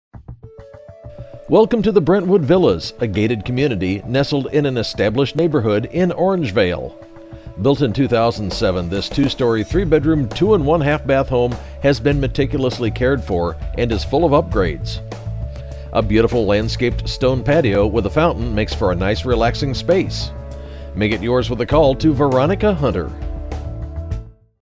6101 Rolkingham Lane Orangevale, CA
6101 Rolkingham Lane, Orangevale, CA

Welcome to the Brentwood Villas, a gated community nestled in an established neighborhood in Orangevale. This home was built in 2007 with standard energy efficient features, including integrated solar roof tiles, saving on energy bills. This two-story, three bedroom, two and one-half bath home has been meticulously cared for and is full of upgrades. A beautiful landscaped stone patio with a fountain makes for a nice relaxing space.